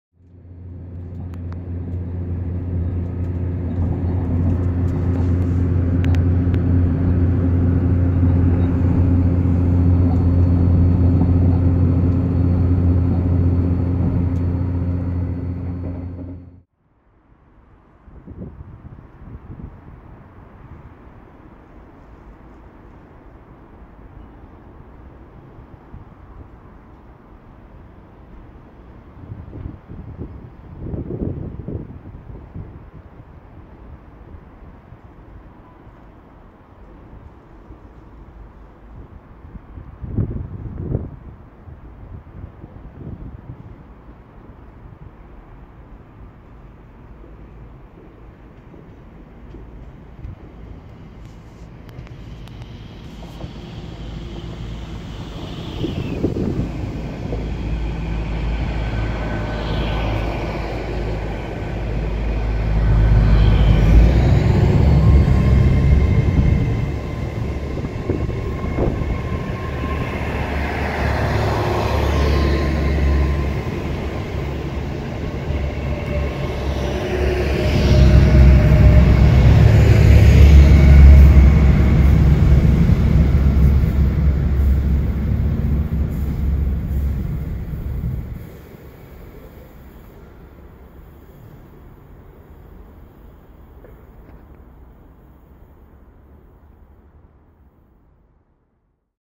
800303: 5X71 12:40 Craigentinny - Inverness | Stirling (27/02/18)
*60FPS* Phone shots of "Not GWR" liveried 800303 on a training run to Inverness for VTEC. Seen out the window at Larbert Junction, then easing through Stirling.